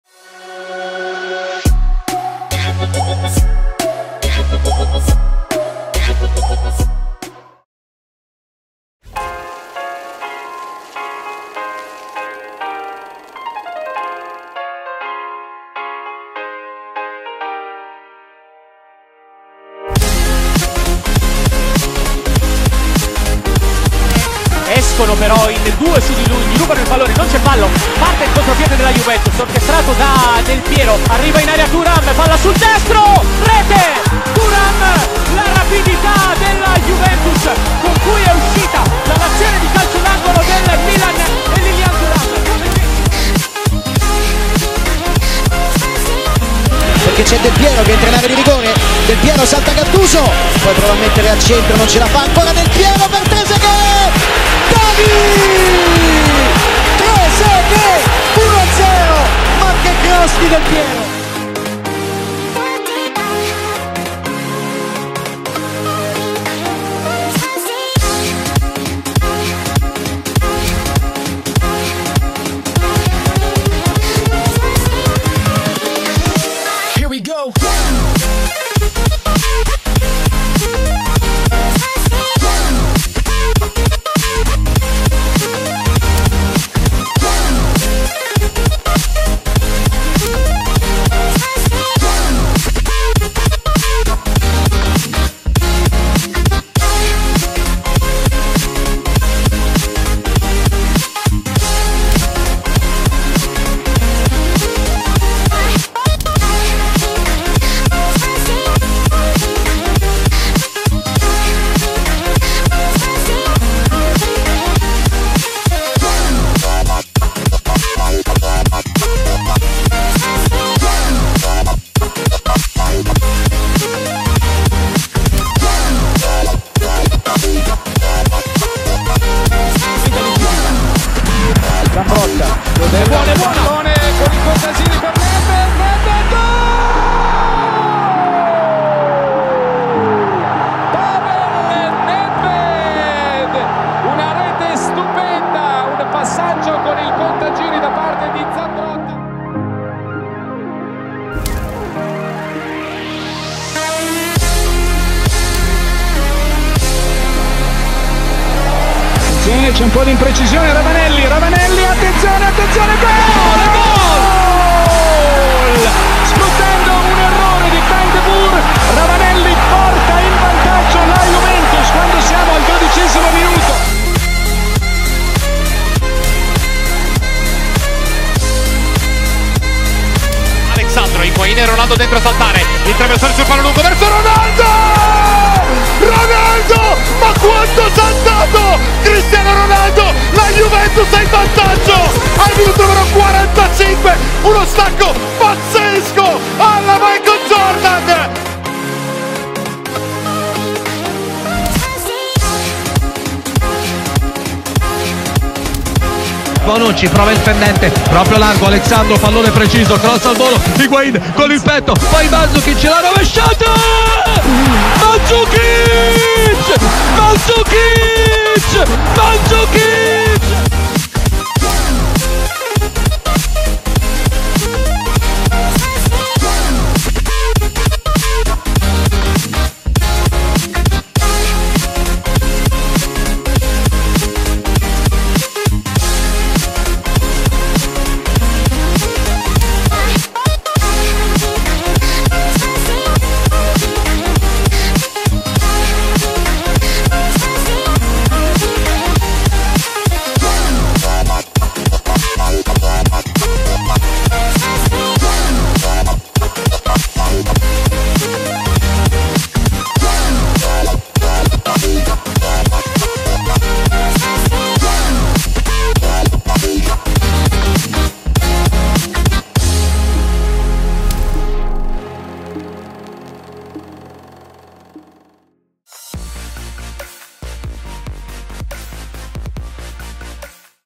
10 GOAL LEGGENDARI che hanno scritto la STORIA della JUVENTUS
10 CAPOLAVORI ASSOLUTI che hanno scritto la gloriosa storia della Juventus. Una raccolta di goal iconici che hanno segnato epoche intere. Zidane, CR7, Thuram, Nedved, Platini, Del Piero.. Quanti campioni!